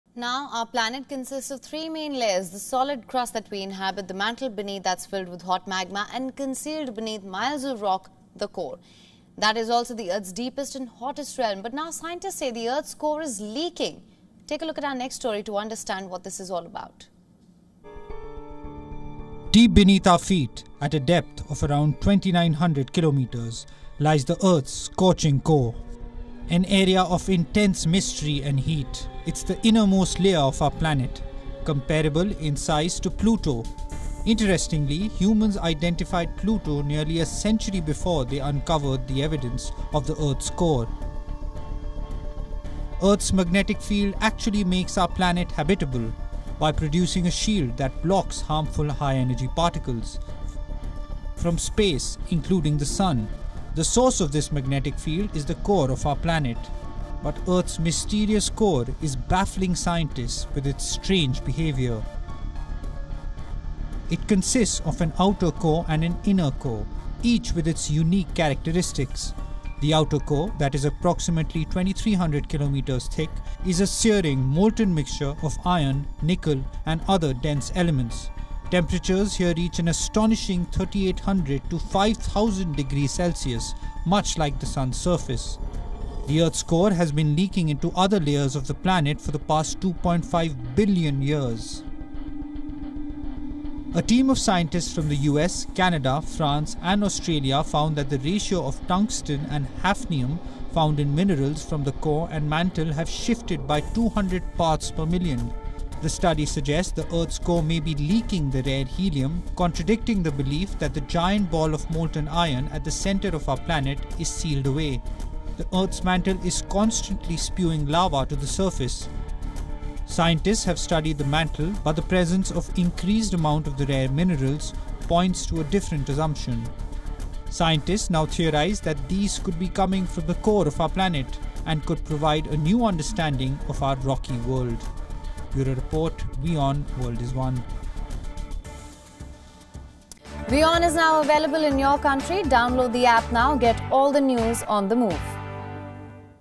Earth's core has been leaking for billions of years | World News | WION
Our planet consists of three main layers, the solid crust that we inhabit the mantle beneath that's filled with hot magma and concealed beneath miles of rock. Watch our report to know more.